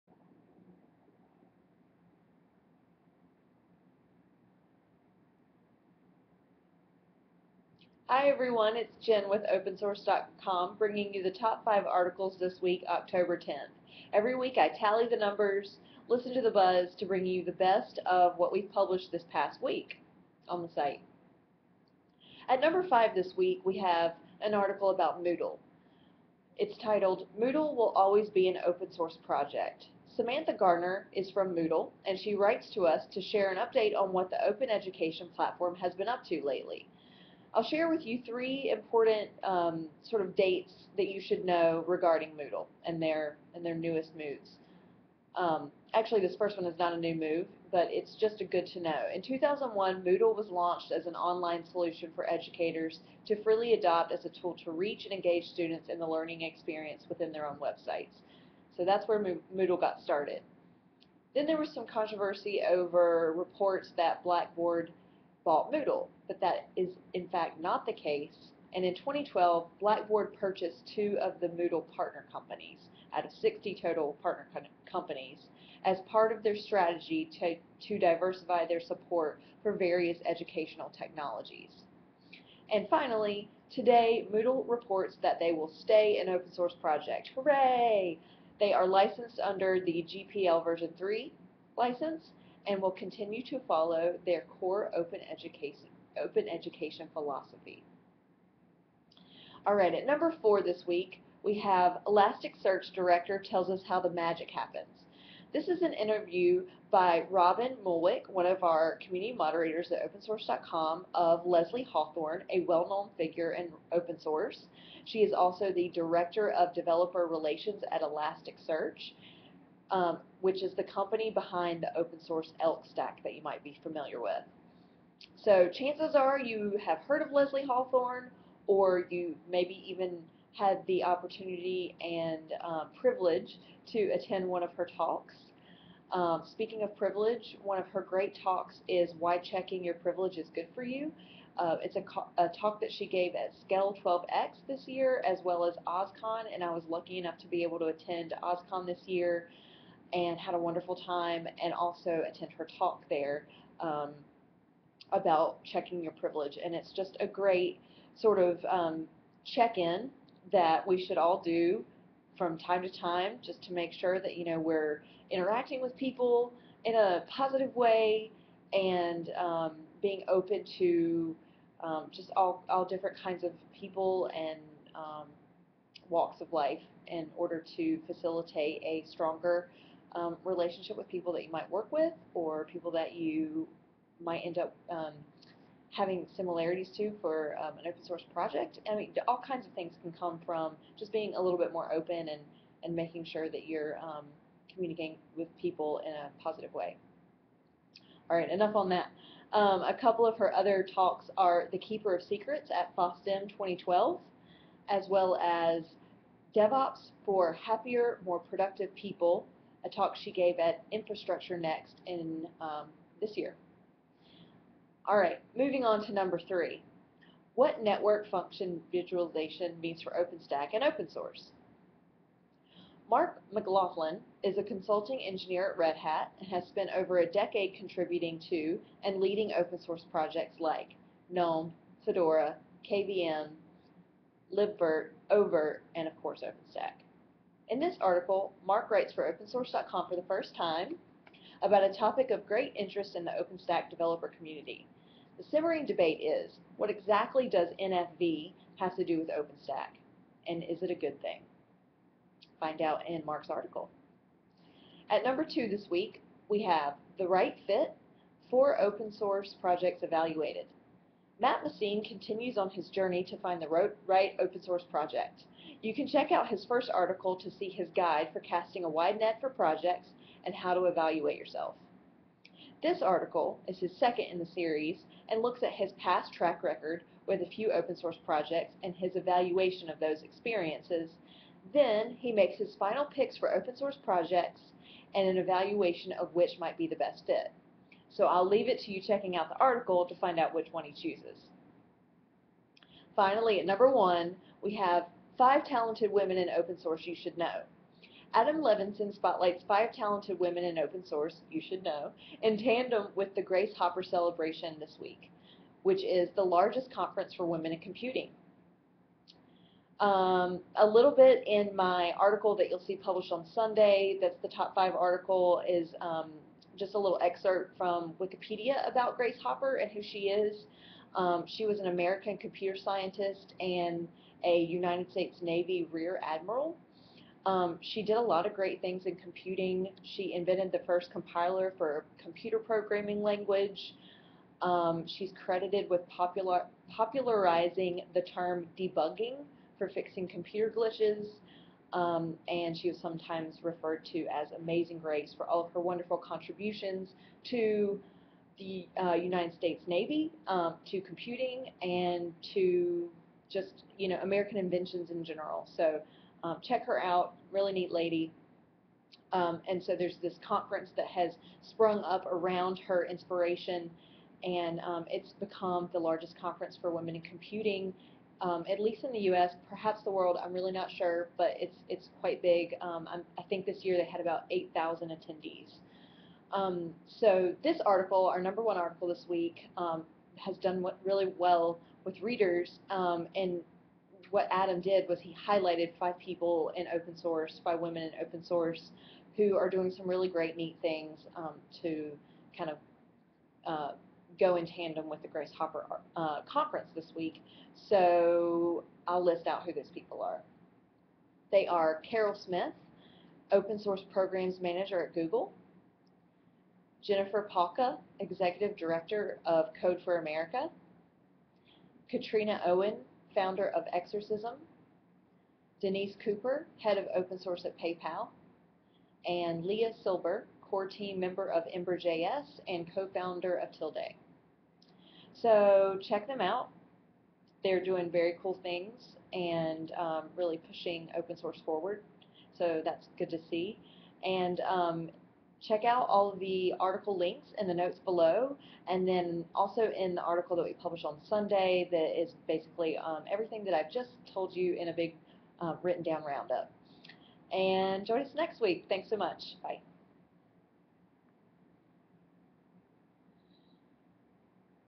Top 5 articles of the week: What open source project are you working on?
Every week, I tally the numbers and listen to the buzz to bring you the best of last week's open source news and stories on Opensource.com, this week October 6 - 10, 2014.

#5. Moodle will always be an open source project

#4. Elasticsearch director tells us how the magic happens

#3. What Network Function Virtualization means for OpenStack and open source

#2. The right fit? 4 open source projects evaluated

#1. Five talented women in open source you should know